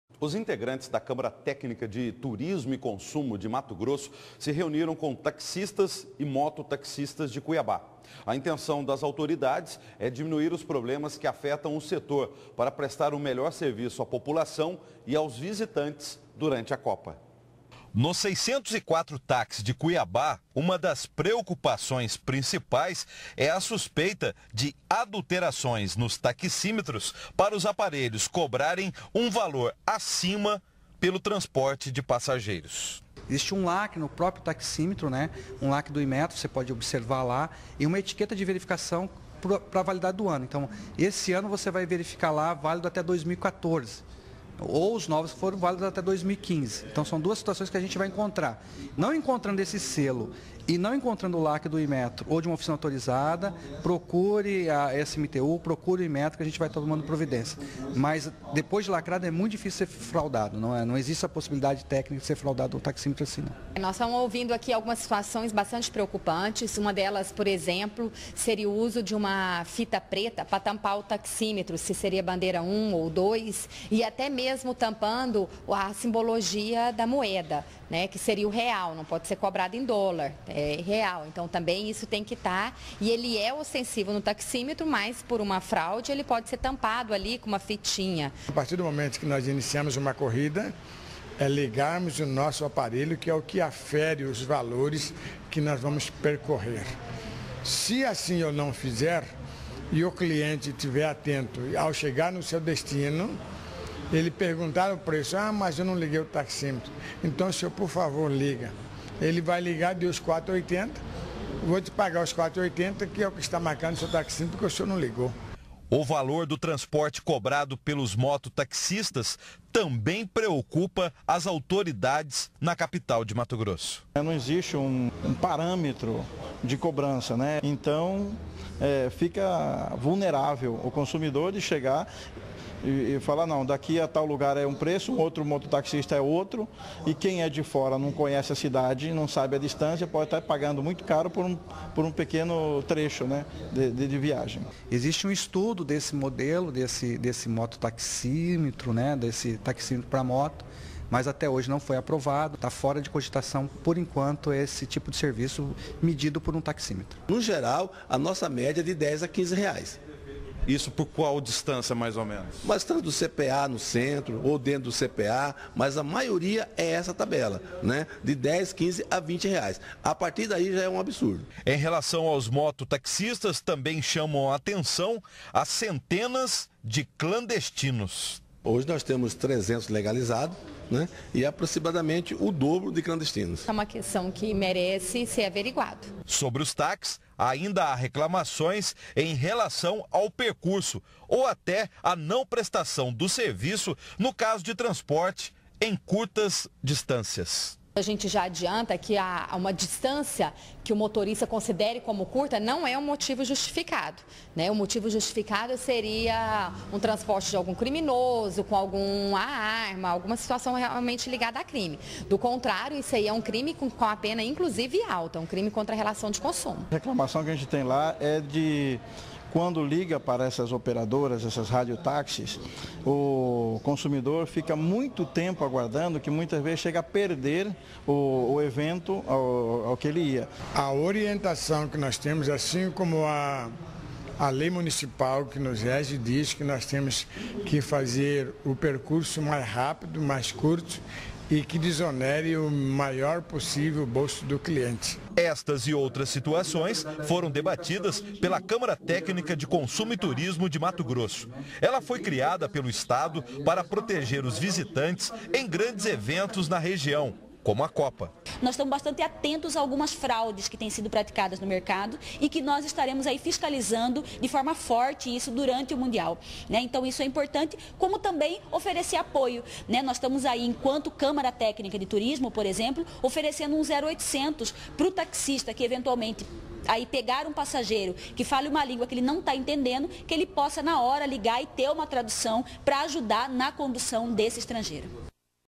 Técnicos do turismo se reúne com taxistas e mototaxistas
A intenção das autoridades é diminuir os problemas que afetam o setor, para prestar um serviço melhor para a população e turistas durante a Copa do Mundo.